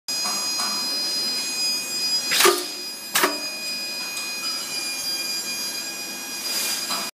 AR-108 , Pneumatic Lead Forming Machine for IPC J-STD-001E
AR-108, Pneumatic Lead Forming Machine---One direction loose radial forming/cutting

    FEATURES

Customized model, Driven by Air With Twin Air-Cylinder, flexible for small and medium lot of components.
Suitable for various types of Radial Lead Components such as Crystal, Capacitor, Resistor etc incorporate with tooling.
Forming and cutting operation in one step.
Compact design and convenience for tooling exchange.

    SPECIFICATIONS

Dimension: L420 x W300 x H200 mm
Weight: 8kgs
Air Source: 5kgs/cm2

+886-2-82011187